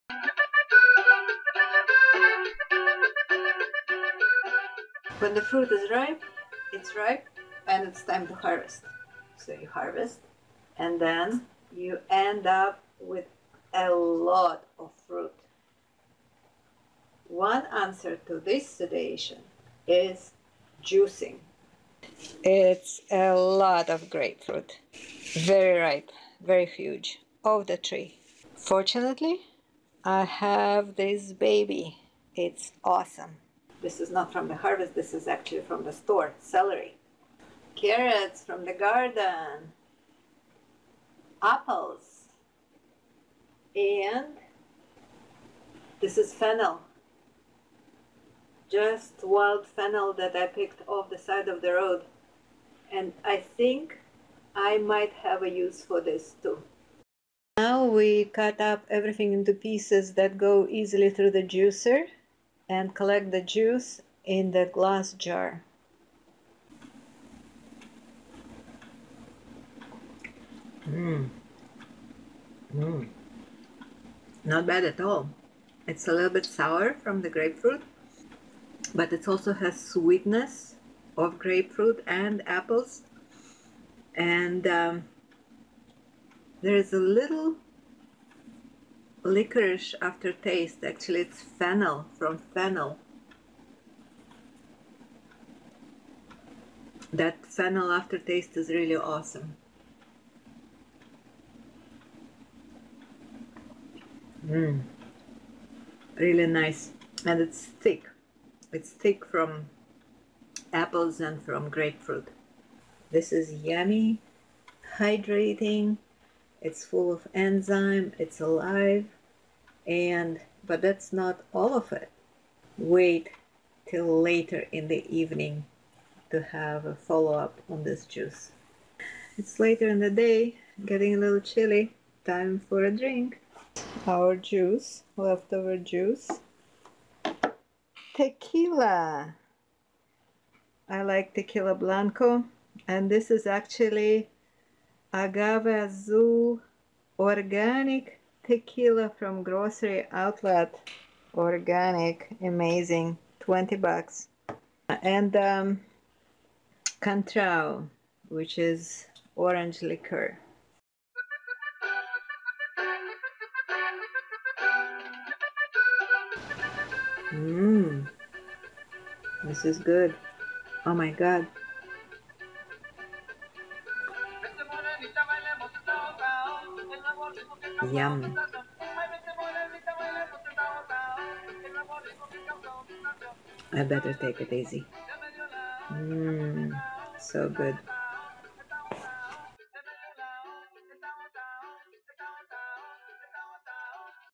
From Grapefruit to Margarita: processing all that fruit after the harvest is SUPER FUN!
When the fruit ripens you better be harvesting and processing right away. Drinking fresh juice is a good super healthy way to use a lot of fruit, and making yourself a glass of chilled Margarita at the end of the day is rewarding.